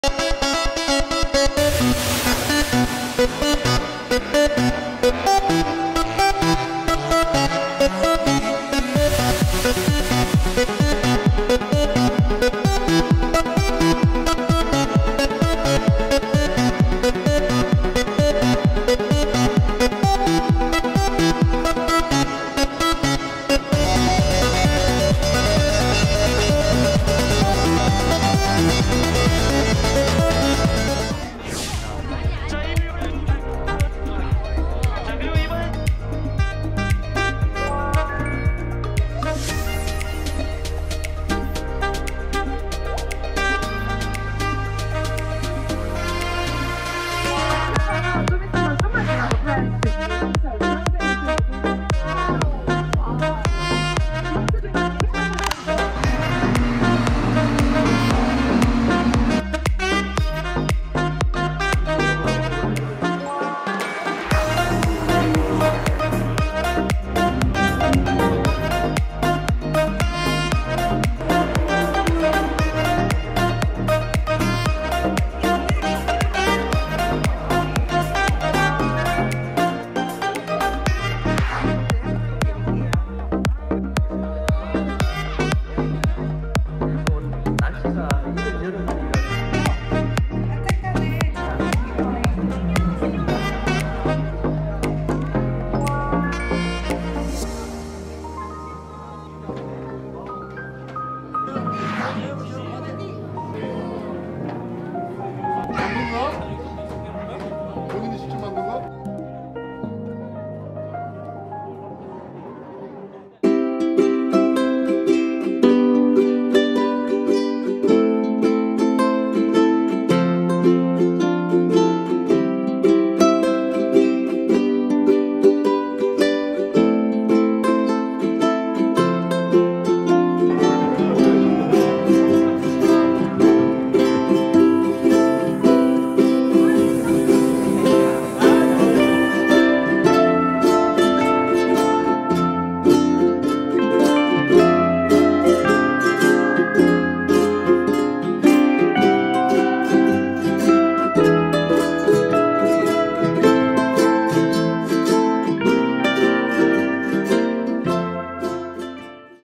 Motor Sports Sponsorship and Children's Program
CJ Logistics is an official title sponsor of the motor racing event 'Superrace Championship'.
Children from local communities are invited to company-sponsored events including motor racing, grid walk and speeding bus programs.